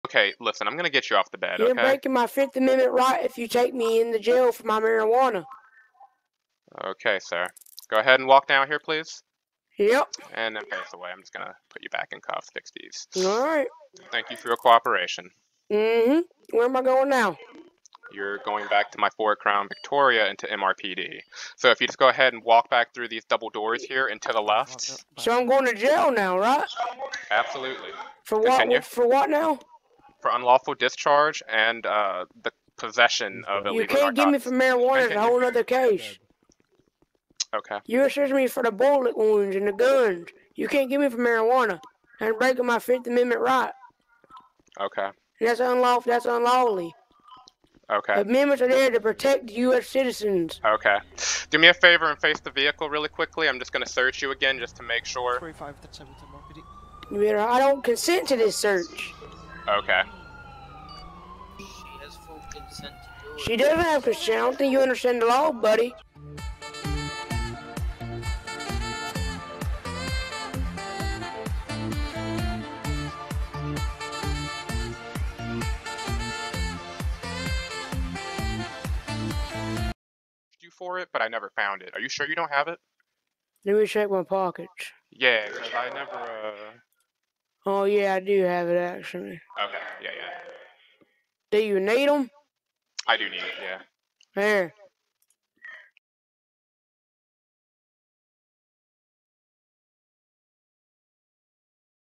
8 Ounces...
So, the video is as described. The suspect seen currently is a middle aged man by the name of "Eric Jenkins" who was detained and taken into police custody by arresting officer 221 | Lieutenant Kuznetsova with the California Highway Patrol for Falsifying a statement, Unlawful discharge of a Firearm in a public area, and Possession of a narcotic over the legal limit (Felonious). The individual sustained that it was "Part of another scene" and that it was a breach of his 5th Amendment right to arrest him for the overzealous amount of Marijuana, however the arrest was made around 6:47 AM on 10/11/2021 for the previously mentioned charges.

The gentleman was sentenced to 20 Months in Prison and $20,000 in citations, fines, and taxes as par California state law.